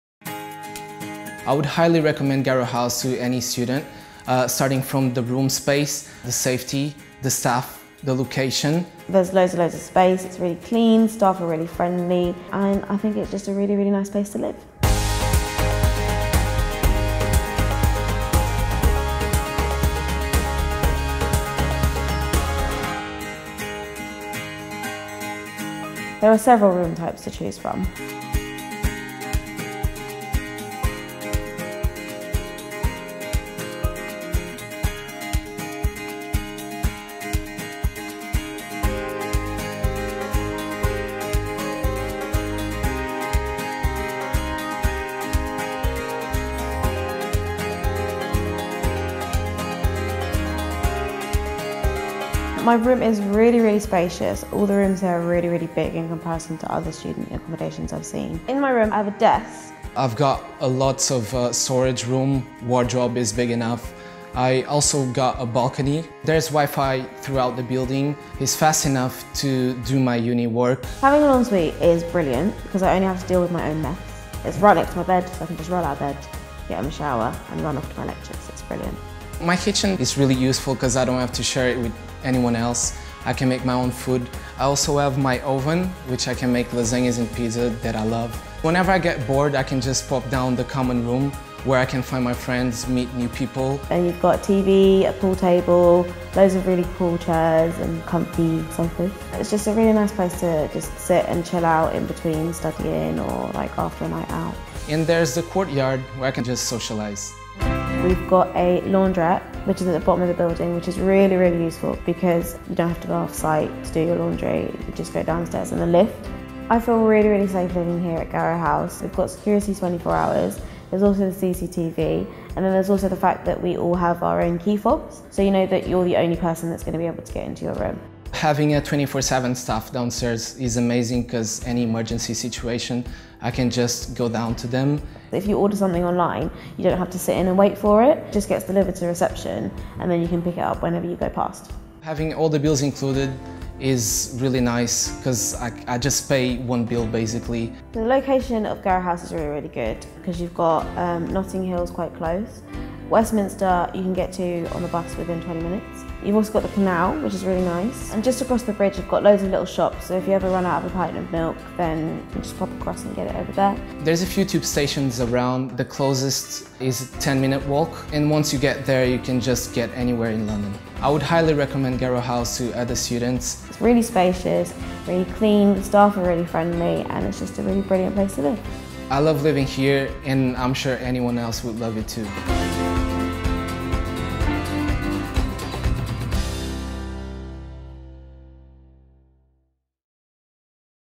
Garrow House, Student Accommodation in London
Garrow House is an impressive, modern student accommodation featuring spacious contemporary studios in a picturesque canalside setting.

Close by you’ll find the world renowned Notting Hill, home to the famous Portobello Road and its antiques market, boutique shops, vintage stores, independent theatres, and on August Bank Holiday the Notting Hill Carnival, Europe’s largest and most vibrant street festival.

Notting Hill was also the location of the eponymous 1999 Richard Curtis smash hit film starring Julia Roberts and Hugh Grant.